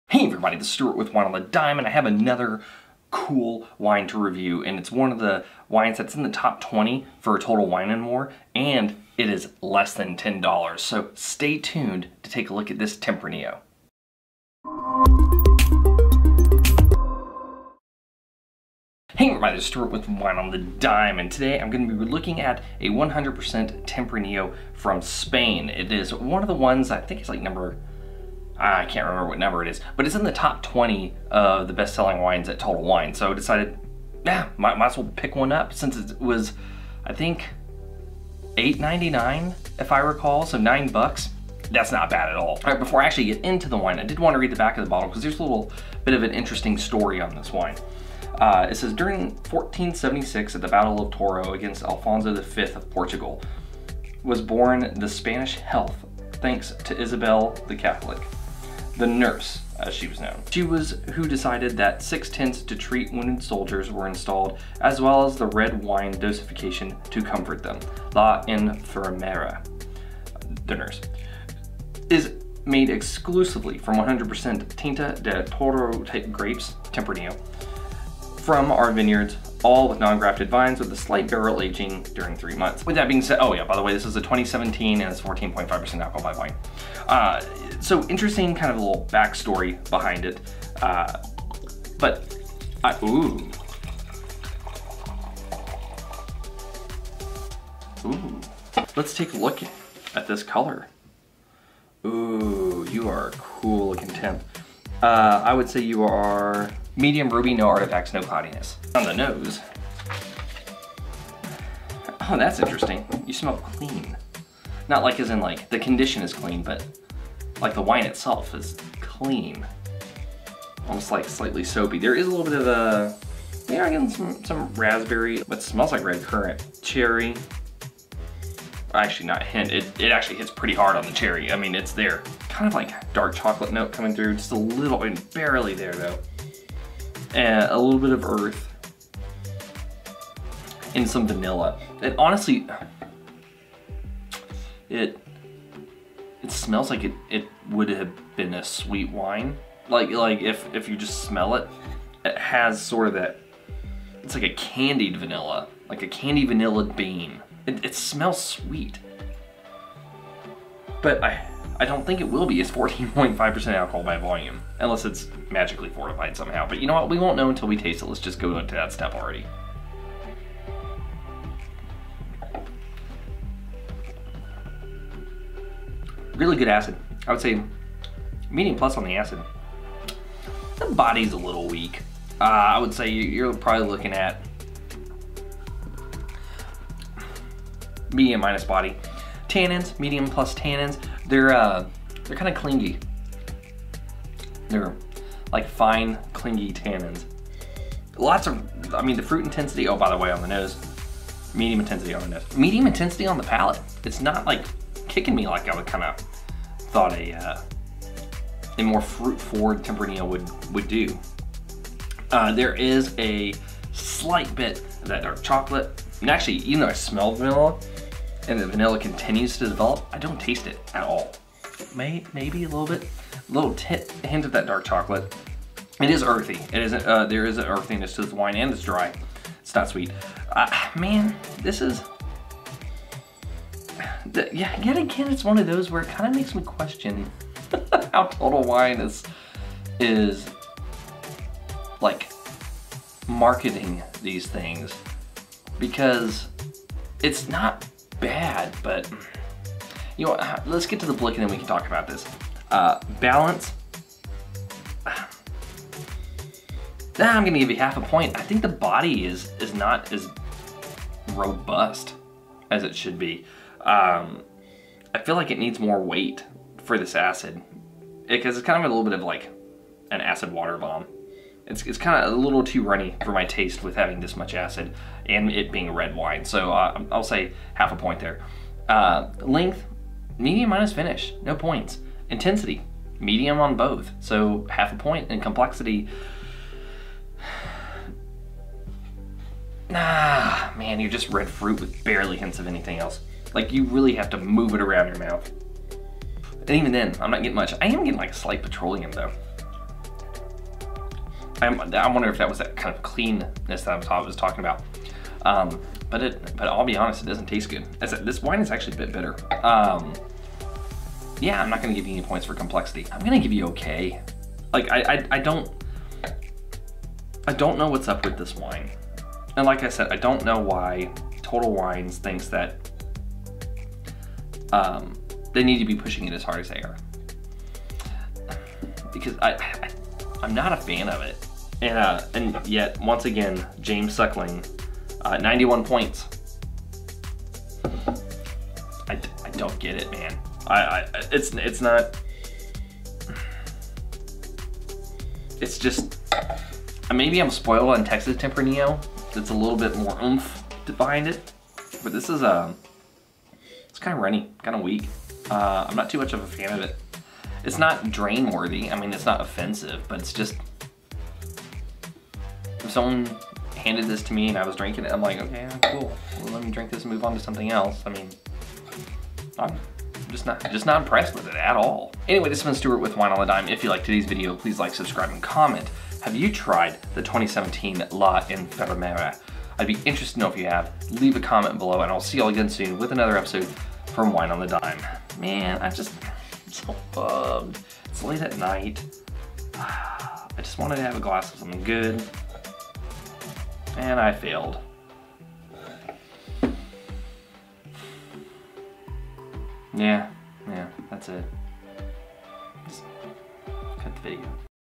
2017 Uro Toro La Enfermera Tempranillo Red Wine Review
Let's end the week with a review of the #4 wine of 2020 for Total Wine & More. Let's take a look at the 2017 Uro Toro La Enfermera Tempranillo.

►Vintage: 2017
►Varietal: Tempranillo
►Region: Toro, Spain
►ABV: 14.5%
►Price: $9

You can help me fuel more content on the channel through...